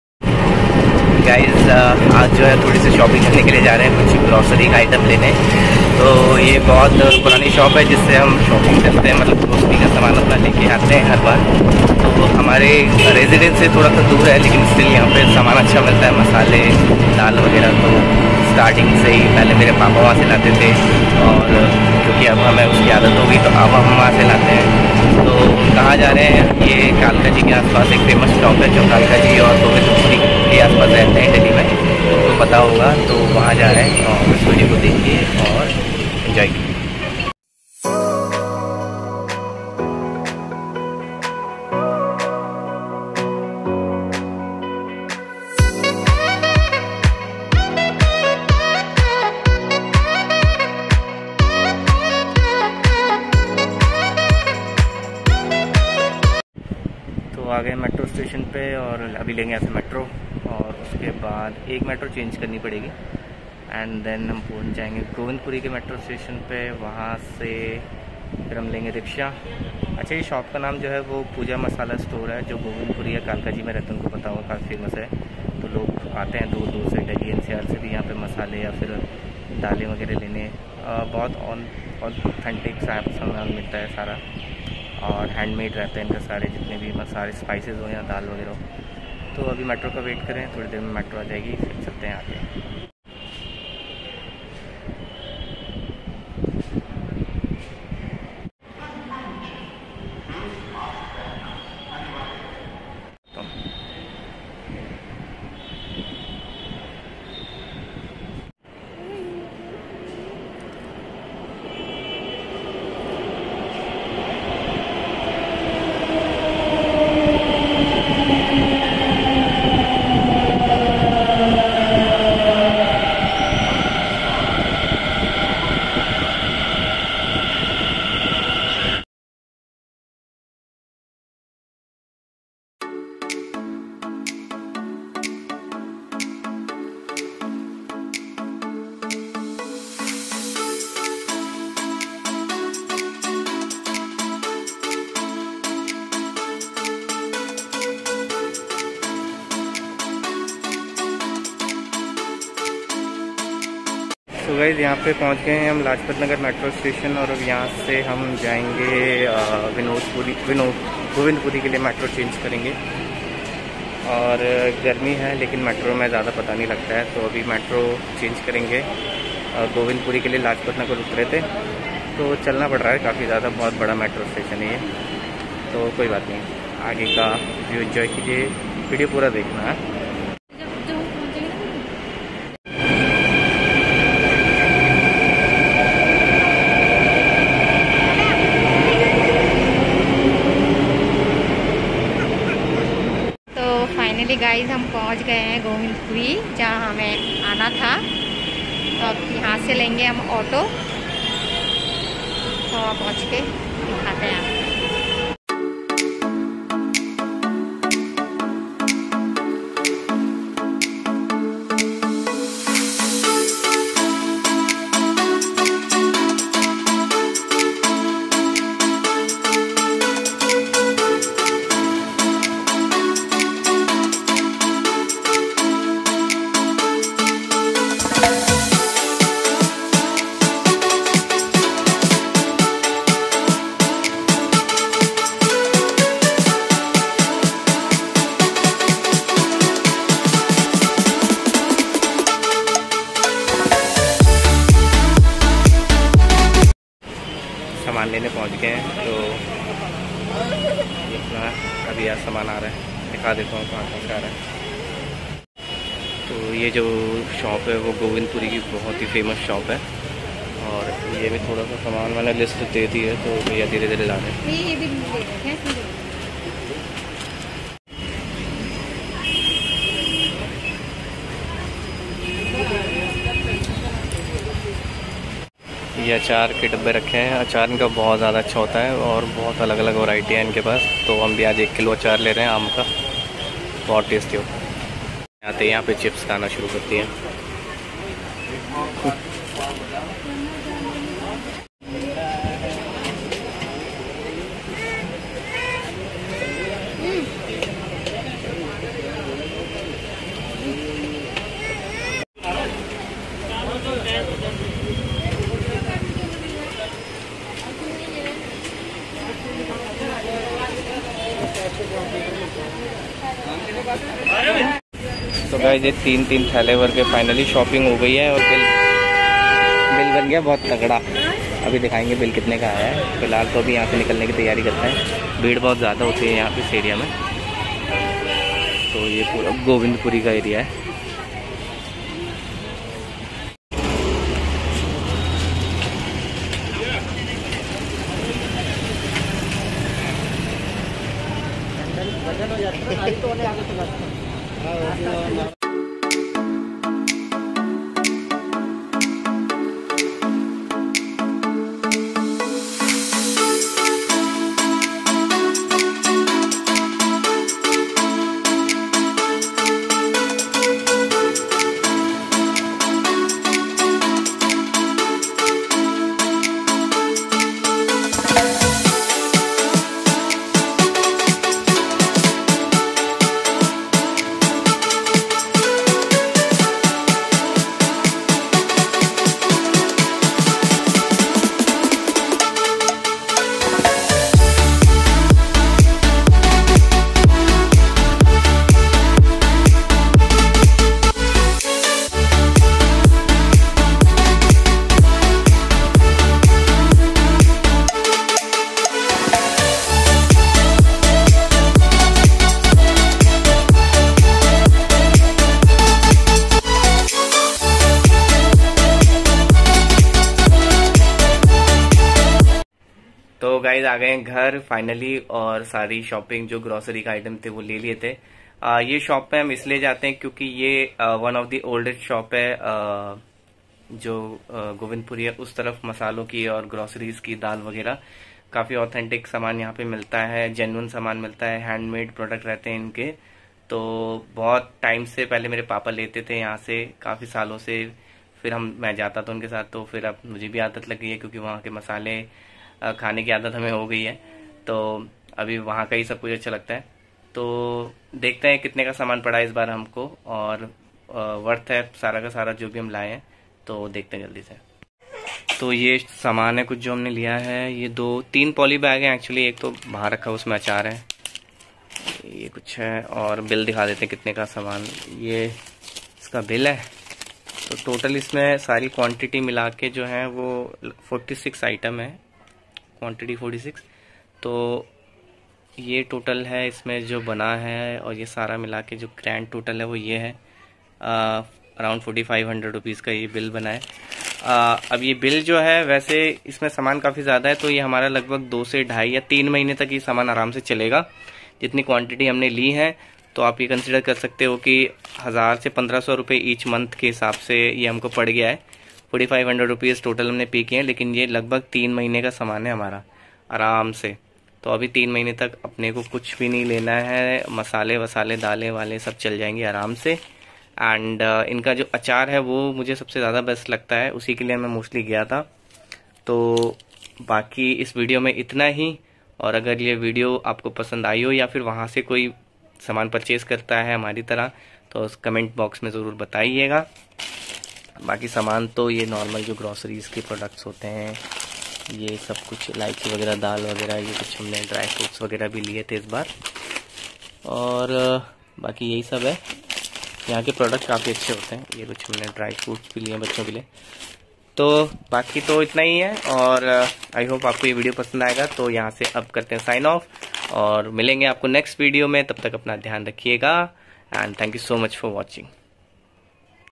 Grocery shopping, south East delhi ki sbse purani shop se, pooja masala store, govindpuri delhi
In this video, we are showing you one of the oldest spices shop of South East delhi, they provides you genuine and hand made pure spices, this is the most famous shop in govind puri delhi. You can reach to govind puri metro station and take sharing e-rickshaw, cost you INR 10 each,

You should once try this show for your grocery shopping,